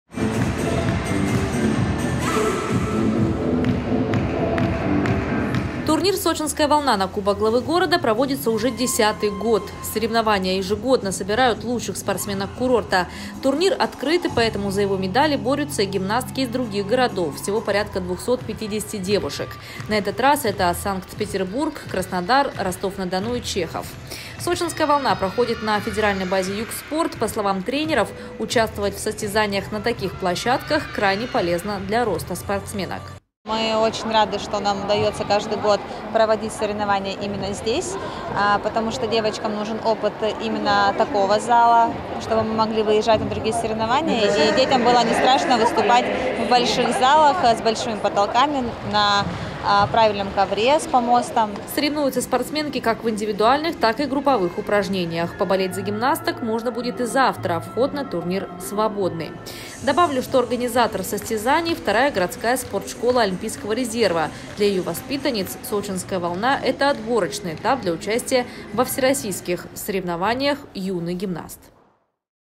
250 гимнасток вступили в борьбу за медали «Сочинской волны». Новости Эфкате
В городе сегодня первый день турнира по художественной гимнастике.

Турнир «Сочинская волна» на Кубок главы города проводится уже 10-ый год. Соревнования ежегодно собирают лучших спортсменок курорта. Турнир открытый, поэтому за его медали борются и гимнастики из других городов - всего порядка 250 гимнасток. На этот раз это Санкт-Петербург, Краснодар, Ростов-на-Дону и Чехов. «Сочинская волна» проходит на федеральной базе «Юг Спорт». По словам тренеров, участвовать в соревнованиях на таких площадках крайне полезно для роста спортсменок.